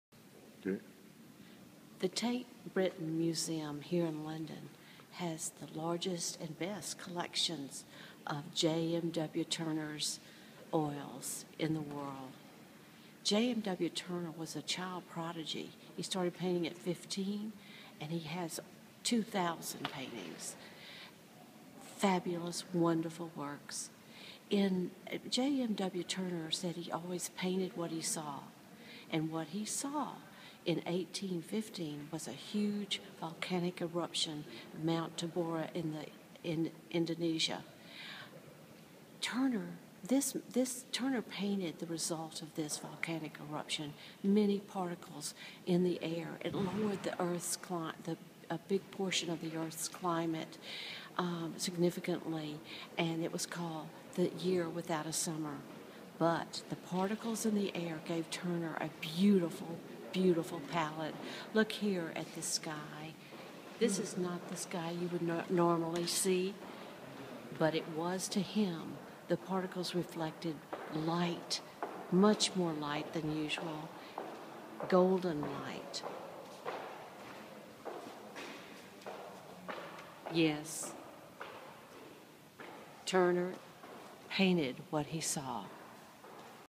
Tate Britain Museum, London and J.M.W. Turner
On location at the Tate Britain Museum in London, England, Kirby discusses the art of J.M.W. Turner.